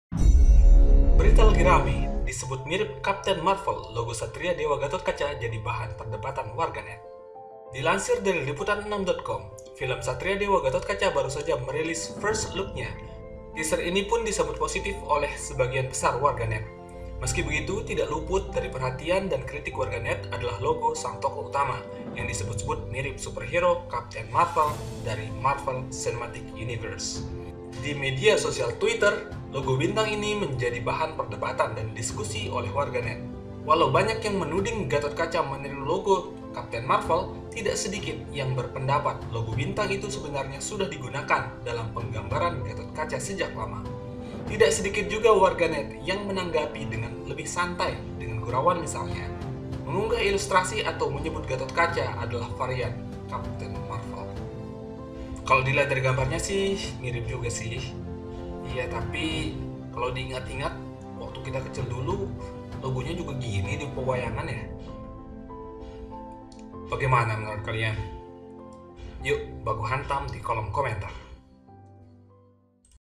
Logo Satria Dewa Gatotkaca Mirip dengan Capten Marvel
Film Satria Dewa: Gatotkaca baru saja merilis First Look-nya. Teaser ini pun disambut positif oleh sebagian besar warganet.

Meski begitu, tidak luput dari perhatian dan kritik warganet adalah logo sang tokoh utama, yang disebut-sebut mirip superhero Captain Marvel di Marvel Cinematic Universe.

Walau banyak yang menuding Gatotkaca meniru logo Captain Marvel, tidak sedikit yang berpendapat logo bintang itu sebenarnya sudah digunakan dalam penggambaran Gatotkaca sejak lama.

Tidak sedikit juga warganet yang menanggapinya dengan lebih santai dengan gurauan, misalnya dengan mengunggah ilustrasi atau menyebut Gatotkaca adalah varian Captain Marvel.
Selamat menyaksikan video Logo Satria Dewa Gatotkaca Mirip dengan Capten Marvel ini. Semoga bermanfaat